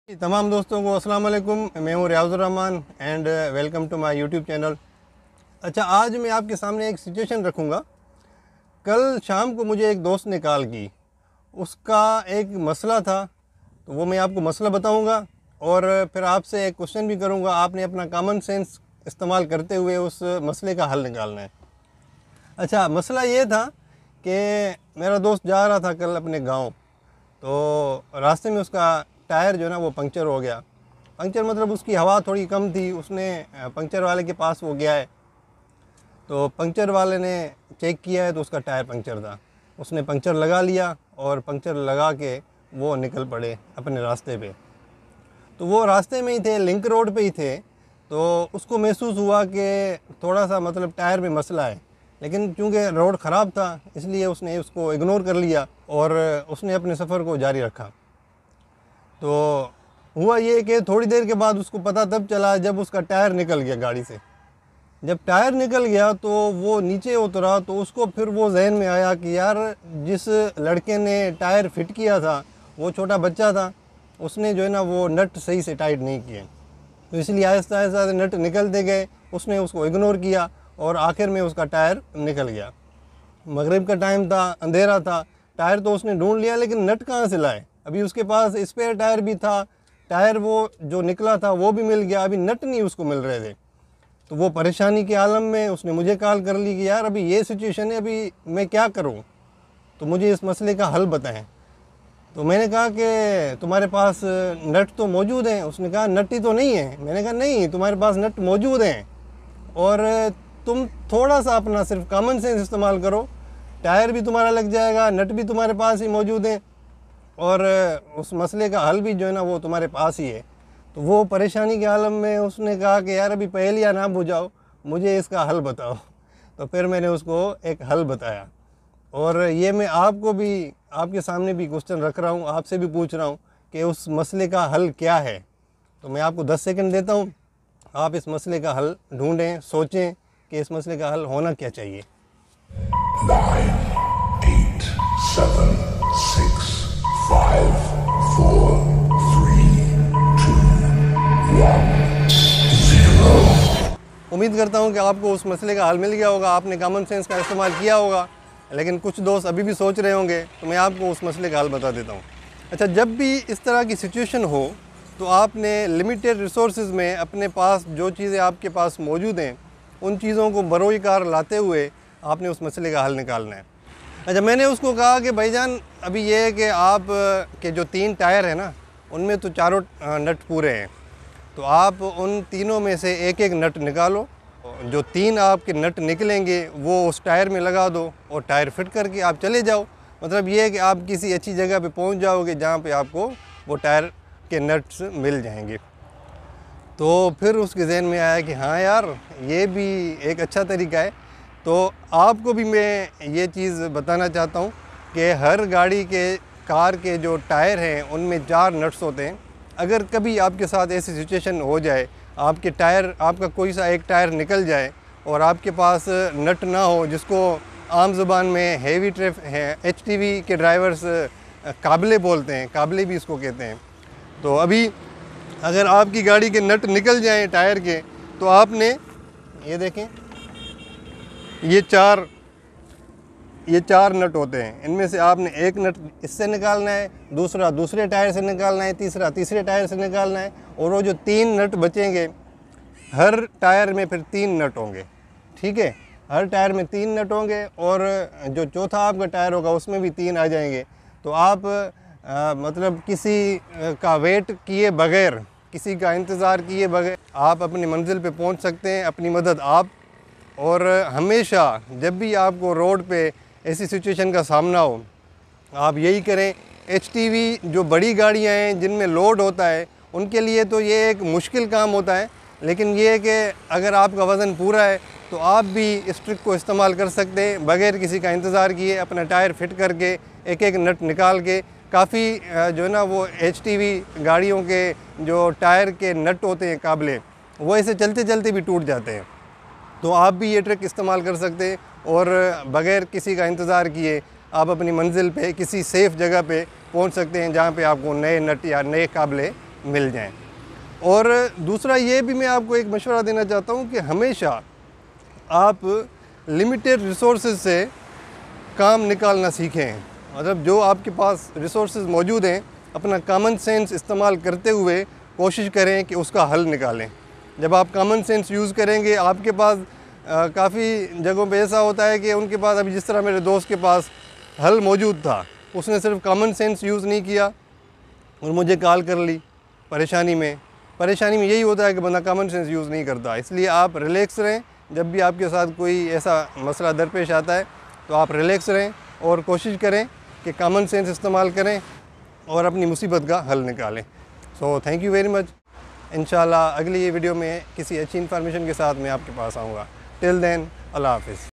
اگر گاڑی کا ٹائر نکل جائے اور نٹ گم ہوجائیں تو کیا کرنا چاہئے What if You Lose your Tyre Nuts
What to do if u lose ur tyre nuts